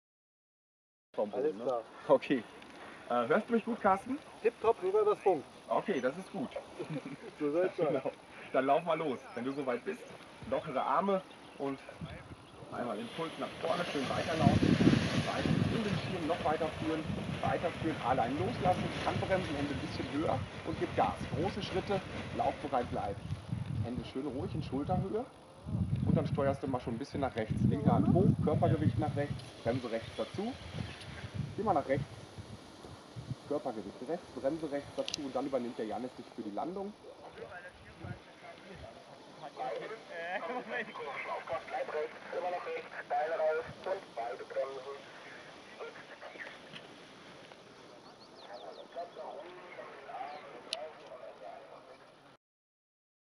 Grundkurs Gleitschirm - erste Versuche am Übungshang
Erster Flug Gleitschirmgrundkurs bei Flugschule Papillon auf der Wasserkuppe im Mai 2015